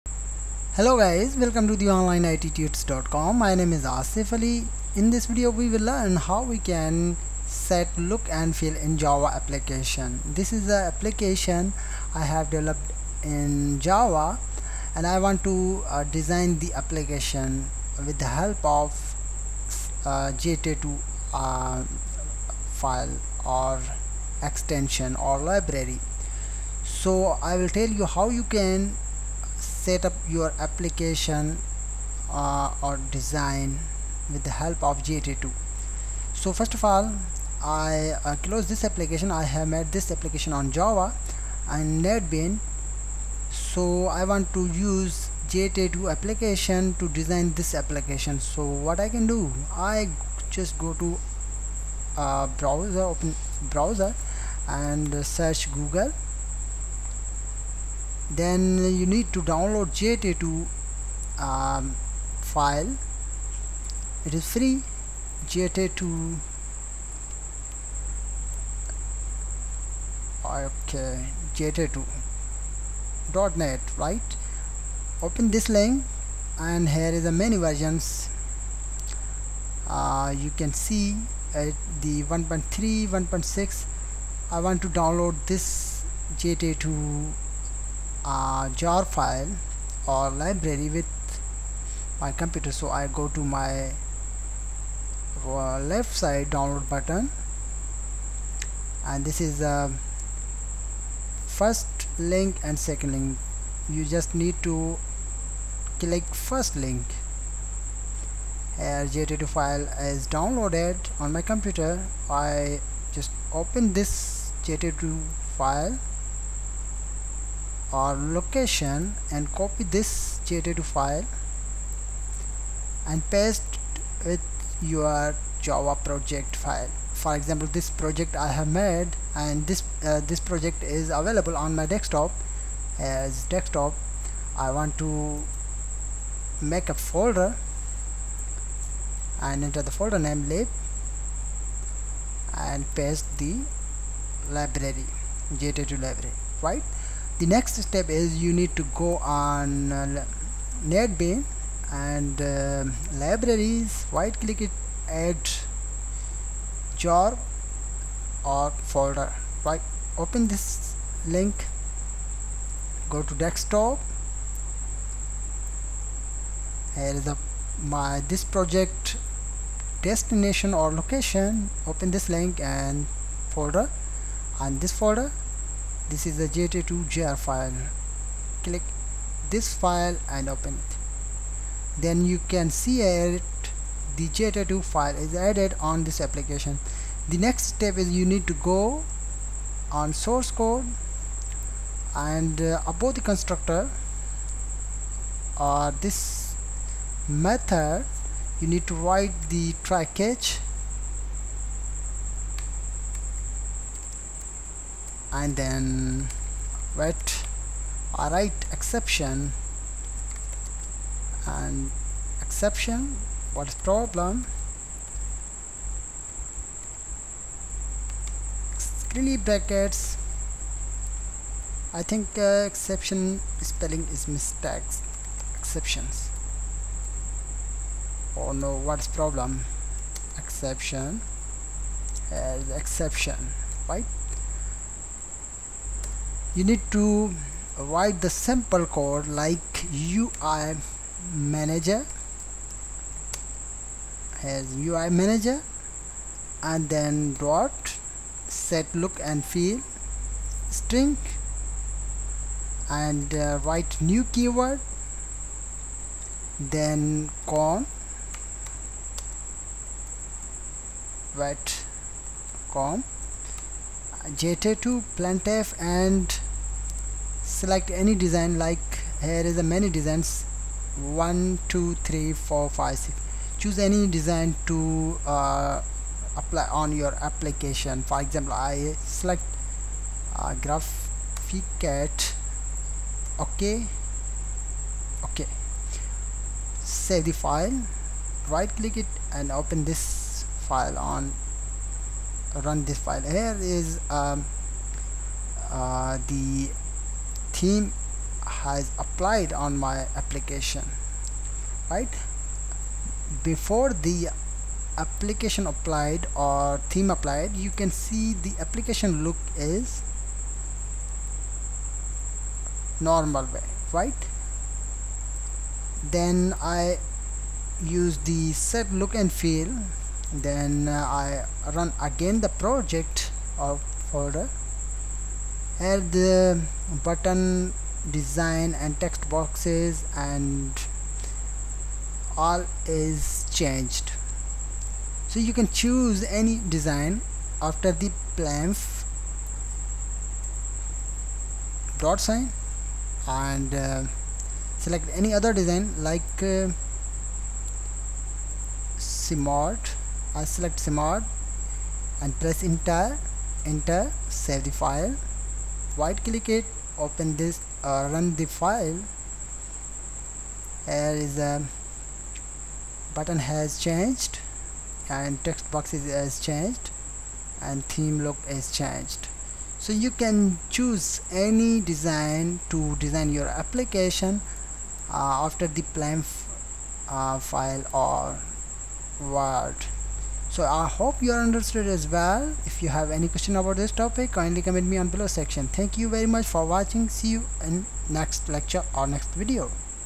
Java Set Look and Feel Tutorial For Beginners
In this tutorial, We Will Learn Java Set Look and Feel Tutorial For Beginners. First of All, you need to create a project in Java, then you need to include items like text boxes, combo box, etc.

Then you need to Download Jtatto File from Jtatoo Official Website. Then you need to Copy Jtatoo Downloaded File and Paste with Project Folder.

Then you need to upload Jtatoo File on Java Project. then you need to write code to use the theme as you like.

So, It is very helpful and beneficial for you, If you have any question/ suggestion about this topic, kindly comment me on below comment section.